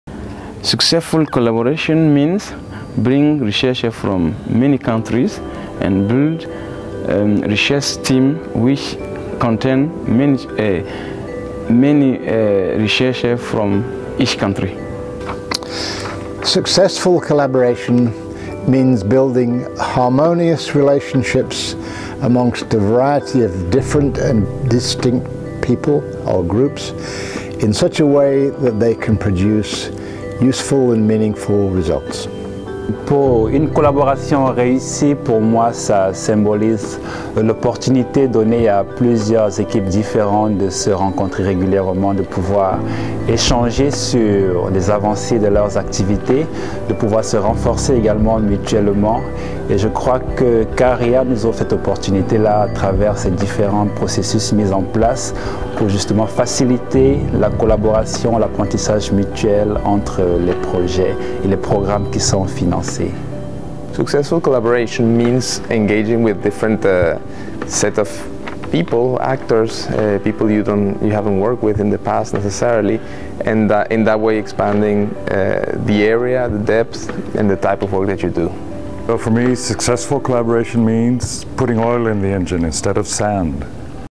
Successful Collaboration means...
At the CARIAA's 2nd Annual Learning Review, we asked some of those
involved what enables successful collaboration.